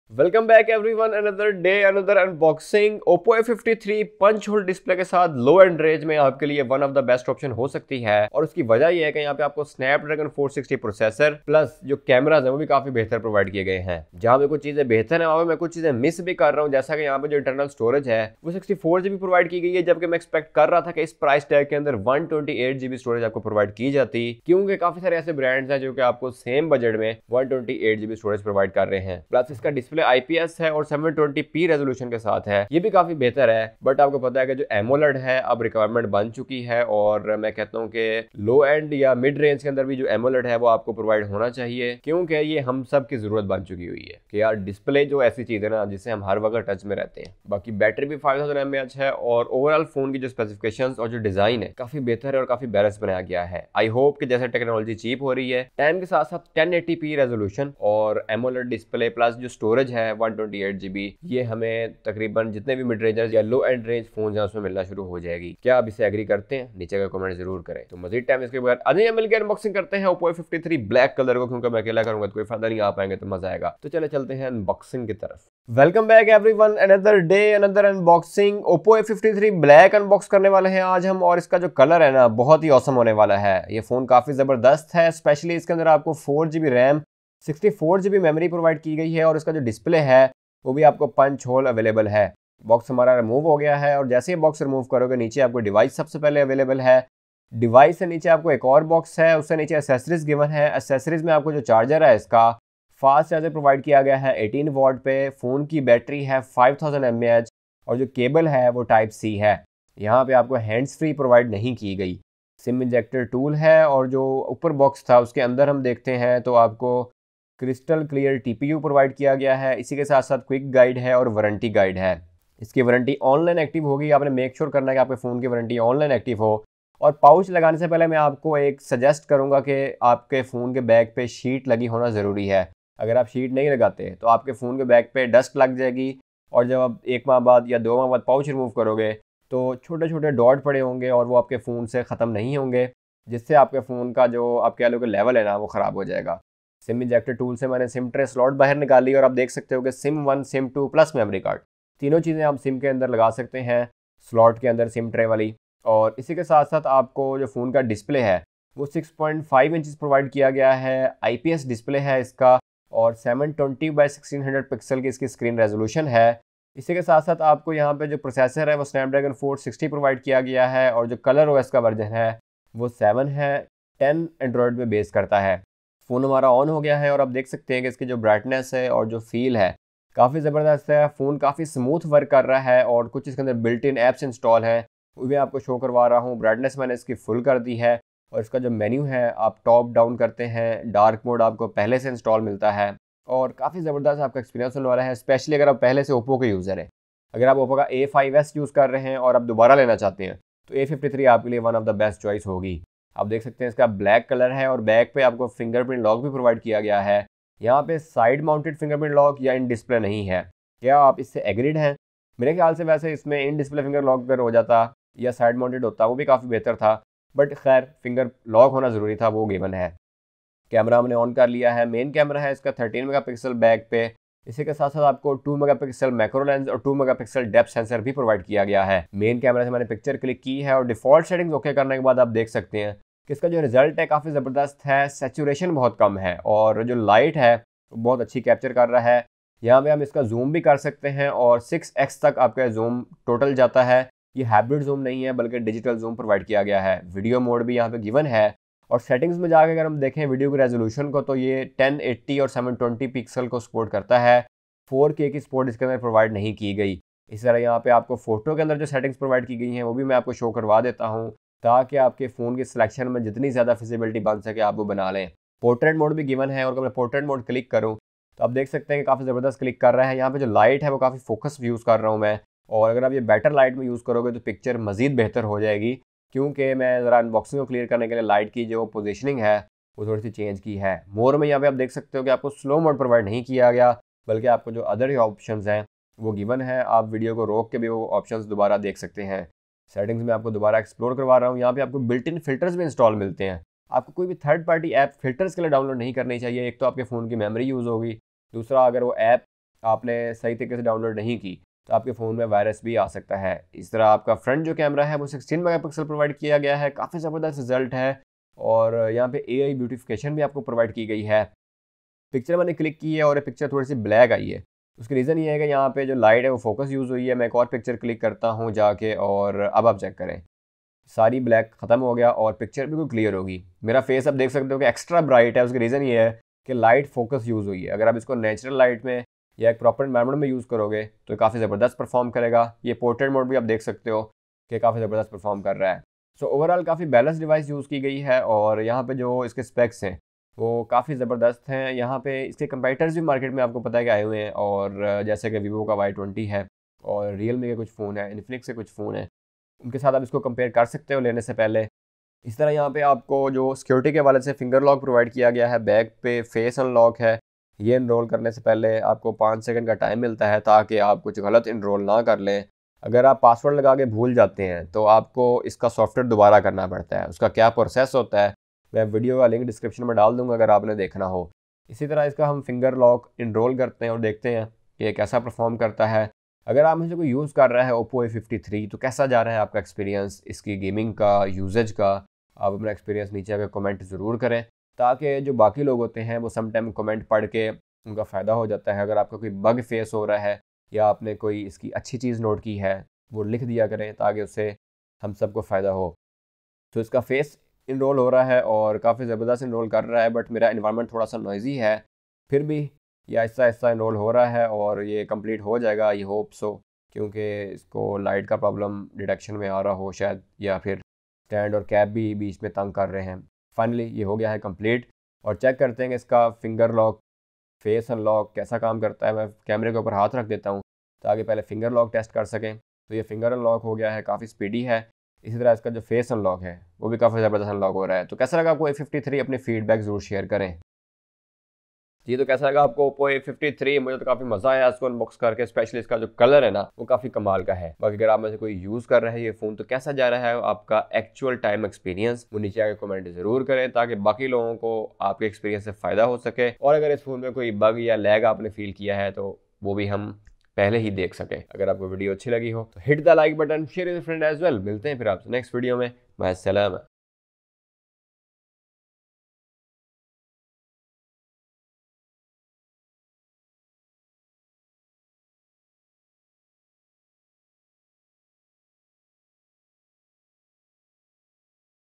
Oppo A53 2020 BLACK UNBOXING AND REVIEW - After 60 Days !!
In this video we will do unboxing of oppo A53. Let's REVIEW OPPO A53 in detail.

★☆★ THE GURU TALKS ★☆★

♠About♠

[TheGuruTalks:]
TECH | Entrepreneurship | Social